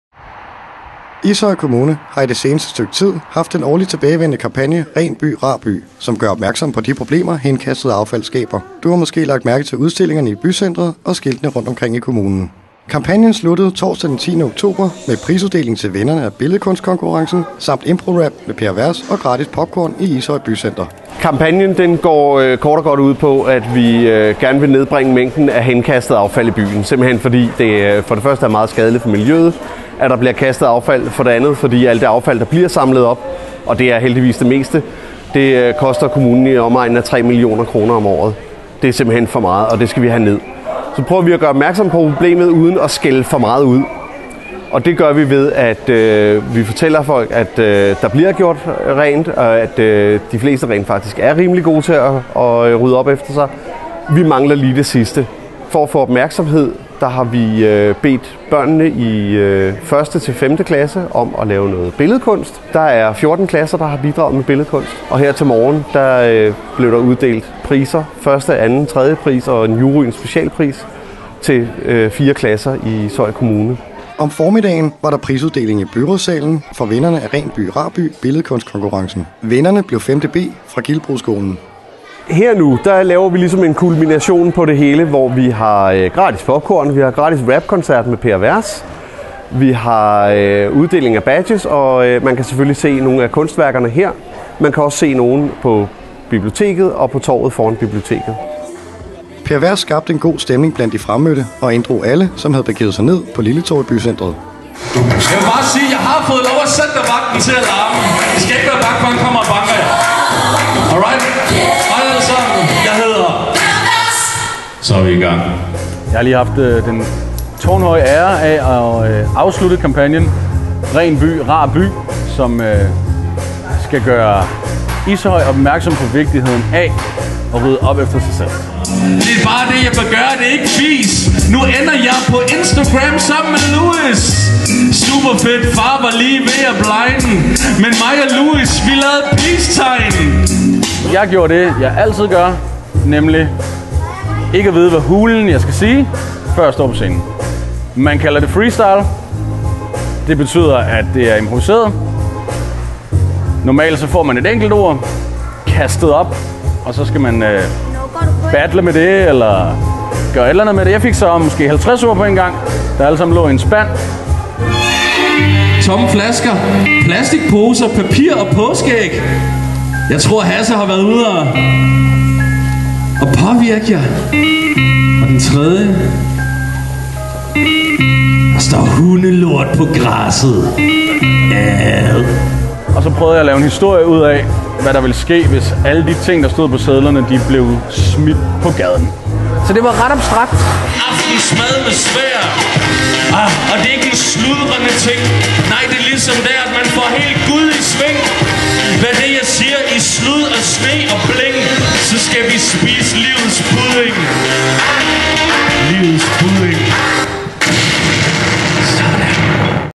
Ishøj - Ren By Rar By - TV-Ishøj
Ren By Rar By kampagnen, som gør opmærksom på de problemer affald skaber. Kampagnen startede mandag den 23. september og afsluttede med prisuddeling, for de folkeskoleklasser, som deltog i billedkunstkonkurrencen og rap med Per Vers på Lille Torv i Ishøj Bycenter torsdag den 10. oktober 2013.